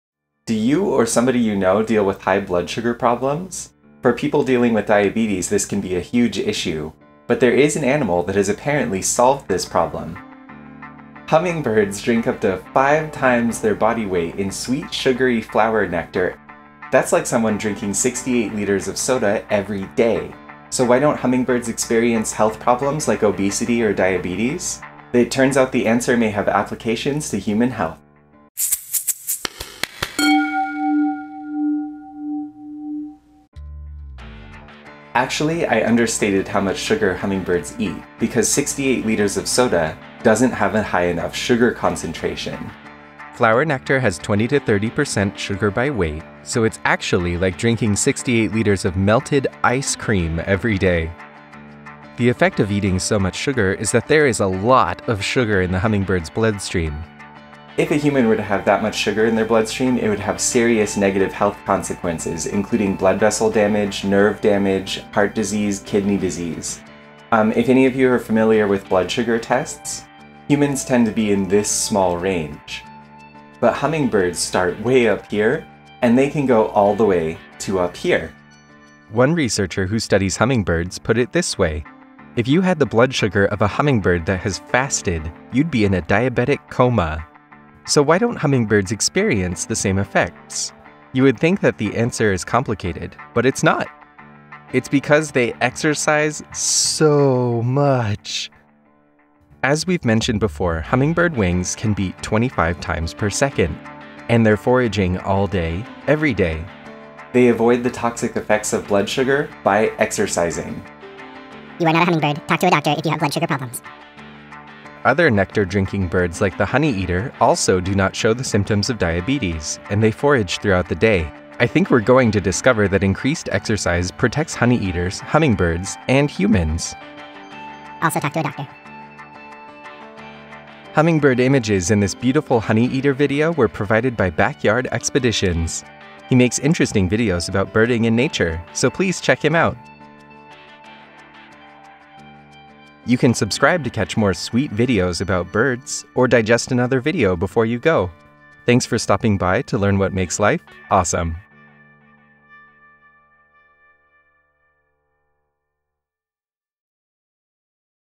Can Hummingbirds Get Diabetes?
Hummingbirds eat a LOT of sugar each day, but does that harm their health in any way?

Hummingbirds eat up to 5 times their body weight in nectar each day!  That's impressive.  And flower nectar is 20 to 30% sugar by weight, so they are eating absurd amounts of sugar each day.  This video investigates whether that harms their health at all, like by causing hyperglycemia.
We discuss how much sugar they eat (a lot), how high their blood sugar can go (really high), and how they avoid the problems associated with hyperglycemia (watch to find out  😀).

This video features photos and video from Backyard Expeditions!  It's a really interesting channel that covers nature and wildlife, so check it out